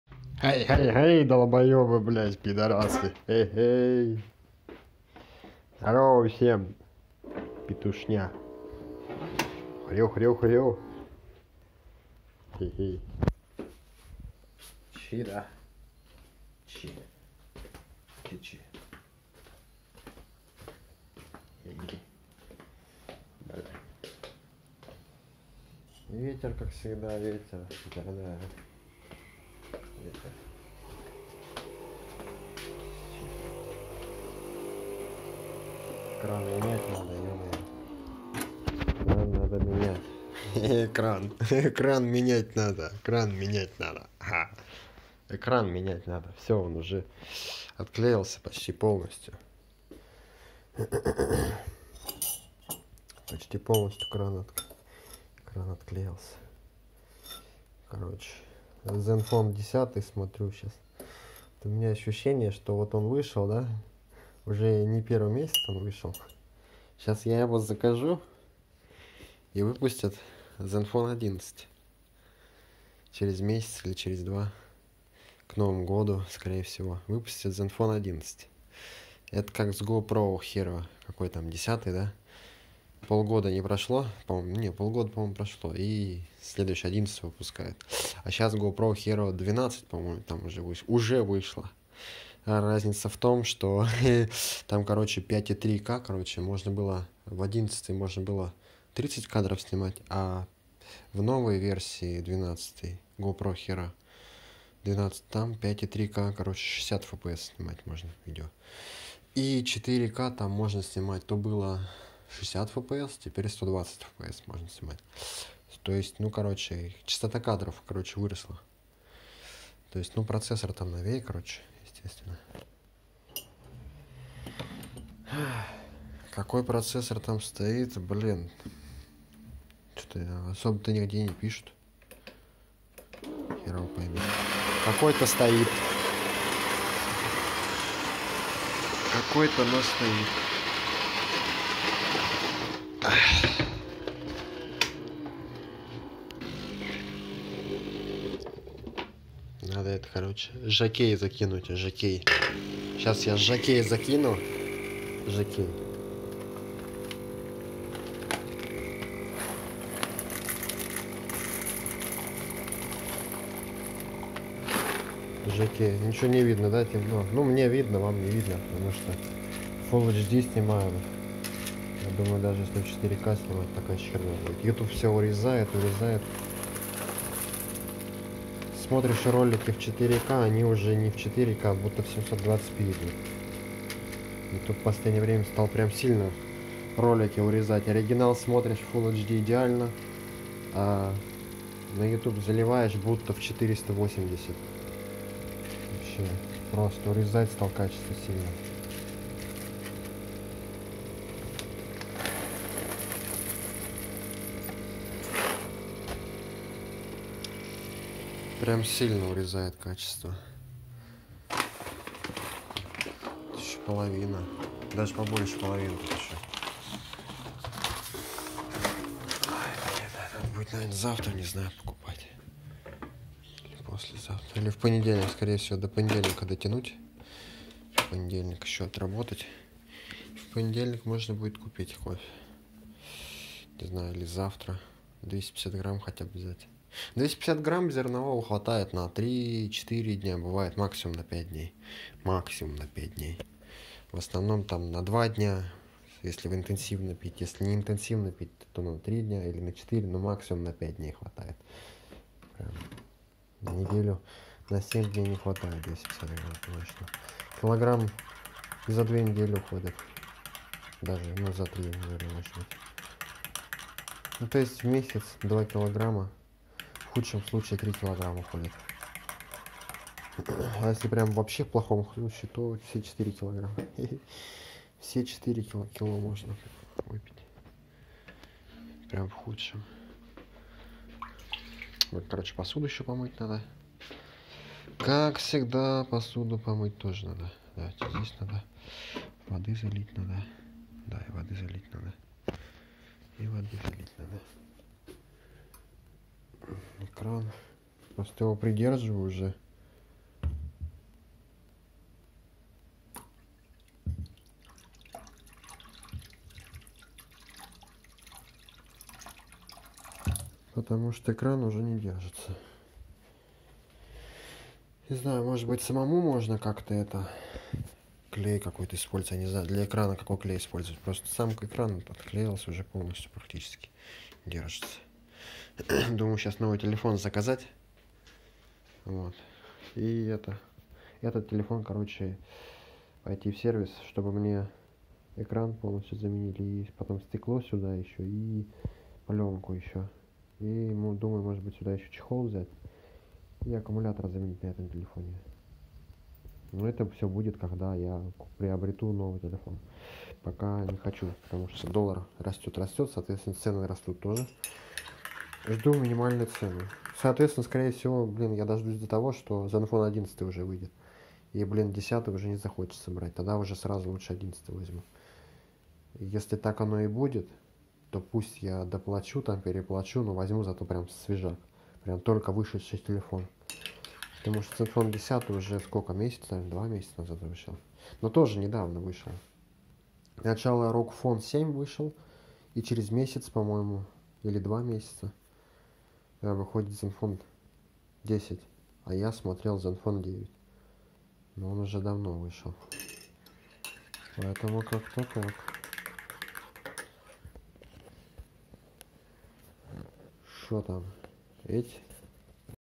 Влог/VLOG/Варим кофе подбираю смартфон/Качество картинки Ютуб урезает сильно/ZenFone 10/GoPro hero12
Что взять из смартфона

 ZenFone 10 либо Asus Rog Phone 7?

GoPro hero 11 или GoPro hero 12?